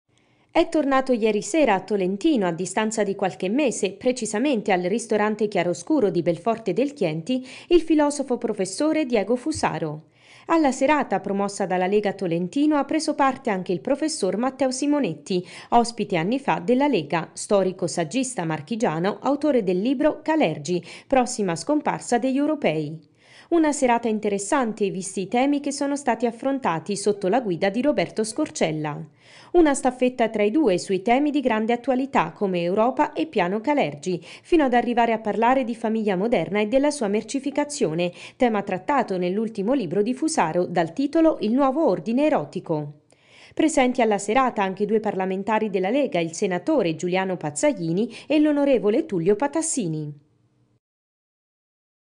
Fusaro e Simonetti ospiti della Lega di Tolentino
La Lega Nord di Tolentino ha invitato di nuovo il filosofo Diego Fusaro per discutere con lo storico Matteo Simonetti di temi di grande attualità, come la mercificazione della famiglia moderna ed il piano Kalergi.